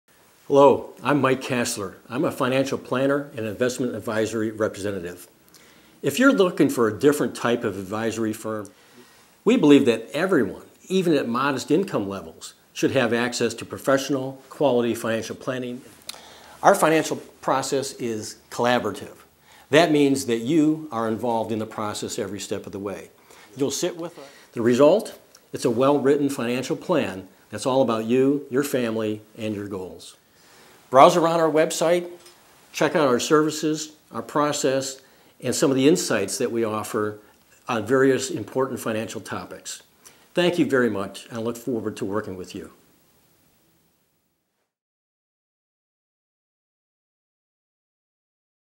Kastler Financial Planning Short
A shorter version of Mike Kastler talks about financial planning